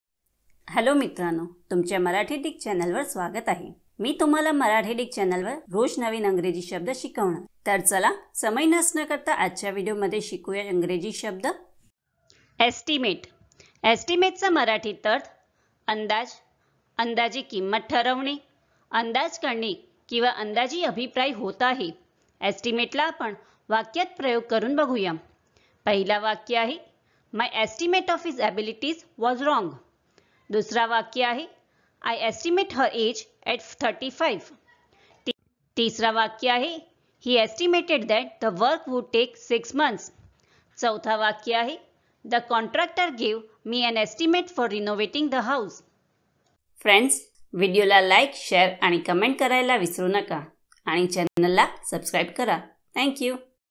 Estimate Meaning in Marathi | Estimate म्हणजे काय | Estimate in Marathi Dictionary
या व्हिडिओमध्ये तुम्हाला मराठीत Estimate चा अर्थ समजेल आणि Estimate चा उच्चारही शिकाल. याचा अर्थ तुम्ही Estimate चा अर्थ जाणून घ्याल तसेच Estimate कसे उच्चारता येईल, Estimate म्हणण्याचा योग्य मार्ग कोणता आहे.

तर Estimate शब्द फक्त एका मिनिटात शिका.

उदाहरणांसह Estimate समजून घेऊ.